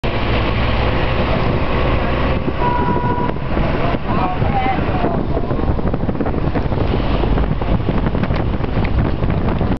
Xian Downtown
recording of xian downtown at night back in summer04 trip